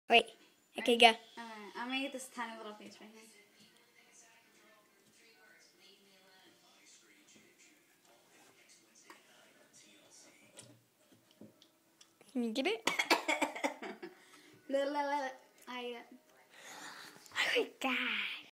Samy eatin toilet paper
Me (samy) & brianna were eating toilet paper after watching my strange addiction